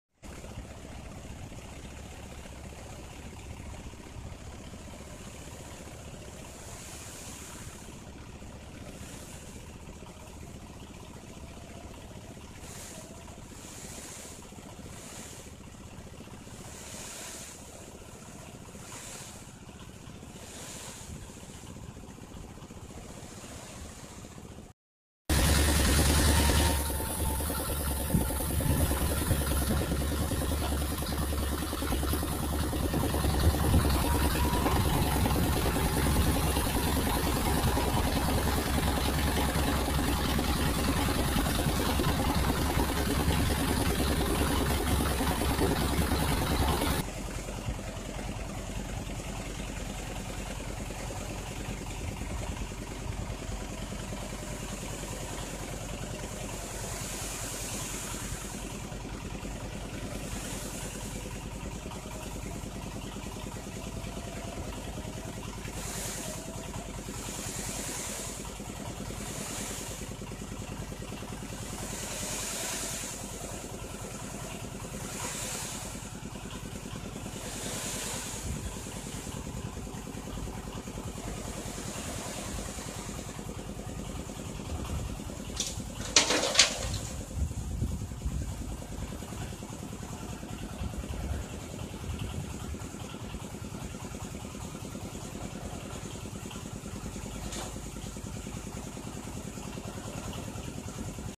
Dump Truk // Truk Pasir Merah Jomplang lagi bongkar Pasir
truk merah merupakan truk kesukaan anak anak